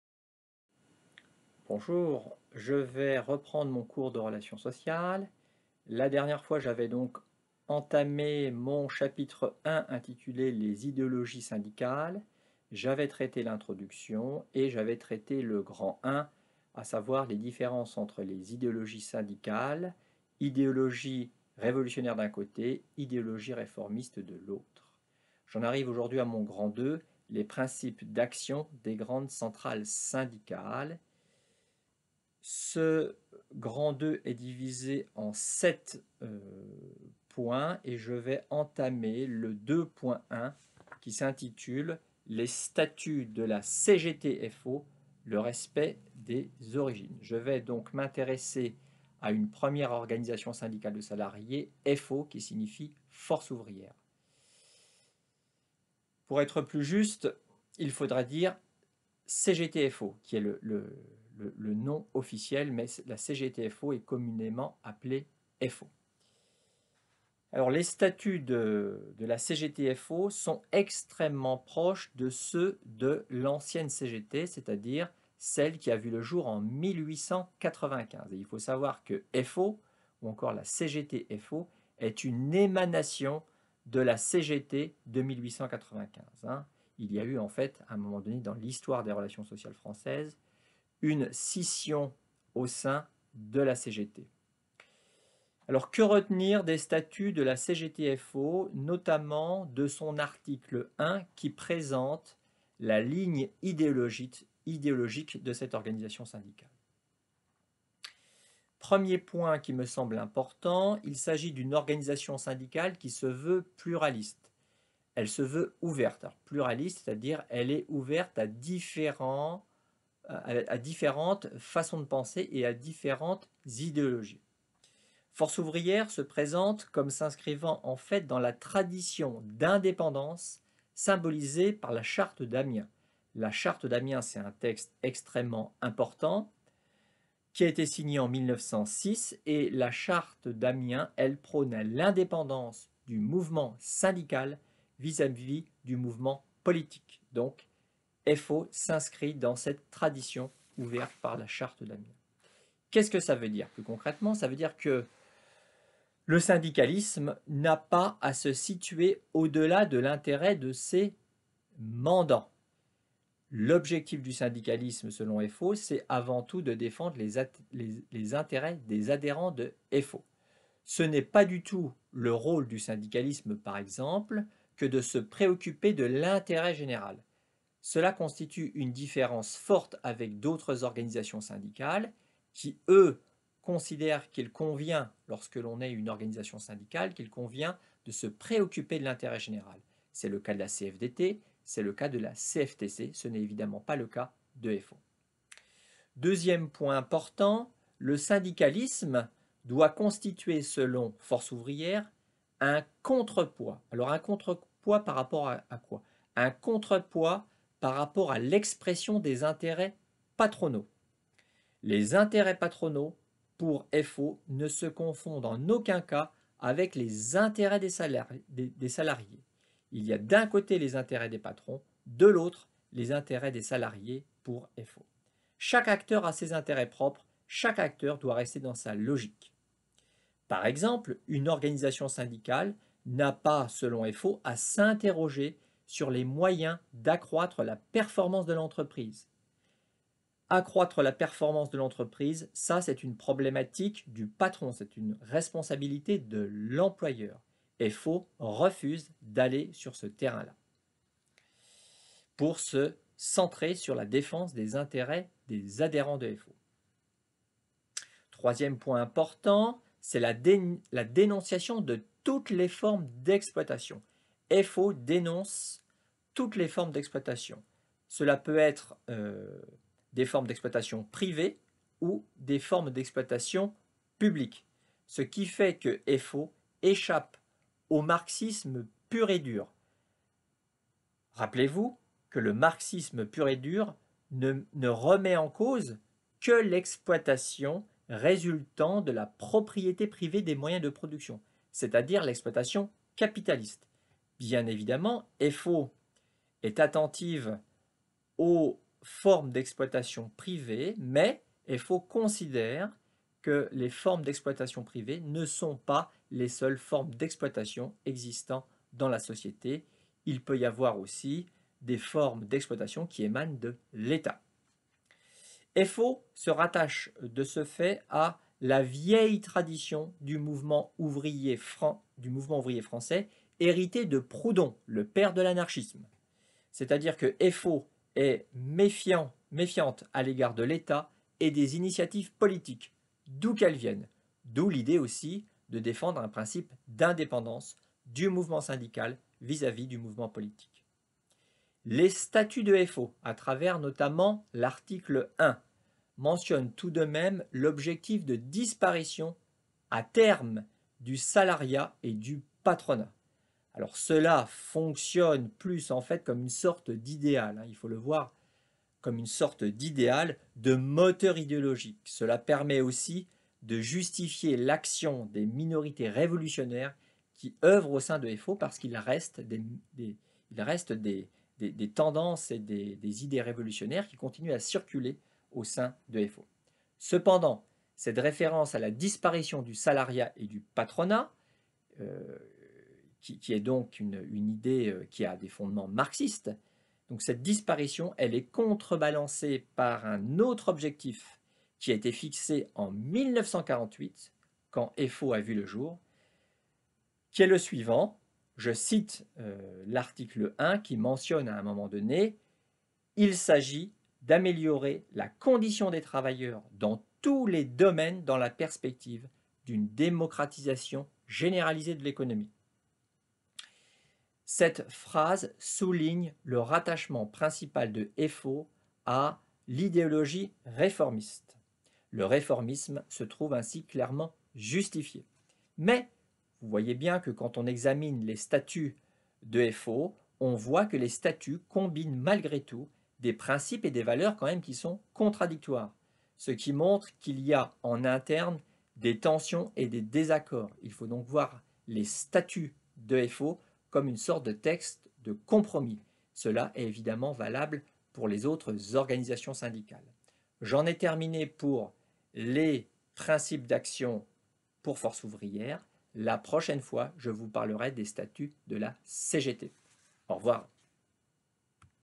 Vidéo 5 La CGT FO
Cette séquence vidéo présente les principes d'action de la CGT-FO.